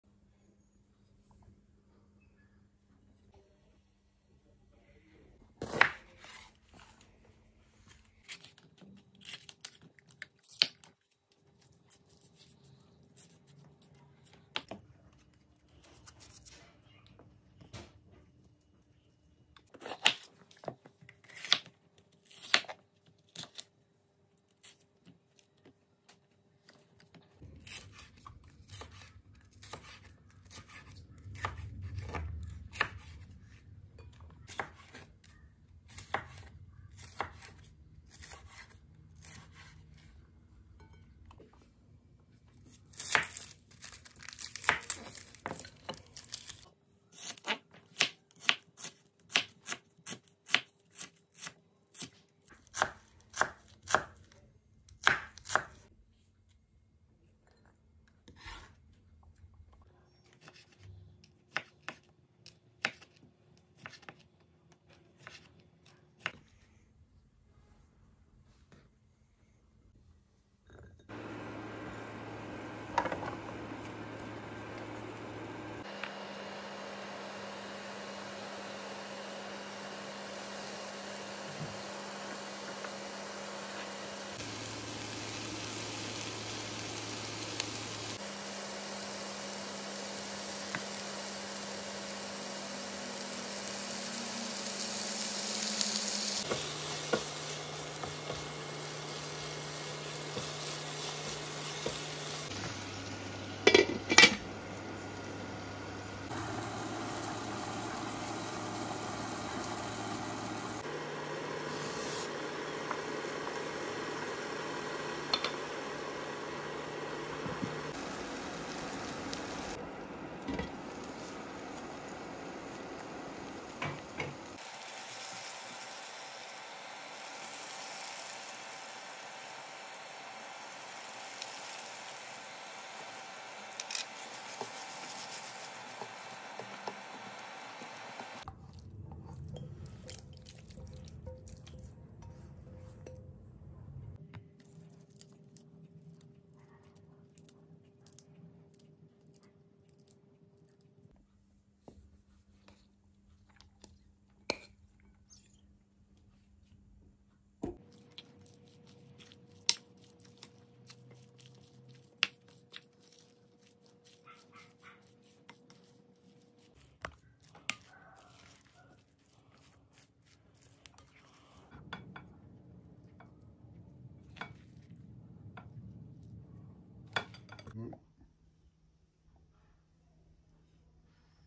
Risotto con peperoni ✨😋
Oggi vi consiglio il "RISOTTO CON PEPERONI E TONNO" se come me amate i risotti lo consiglio
assolutamente, semplici ingredienti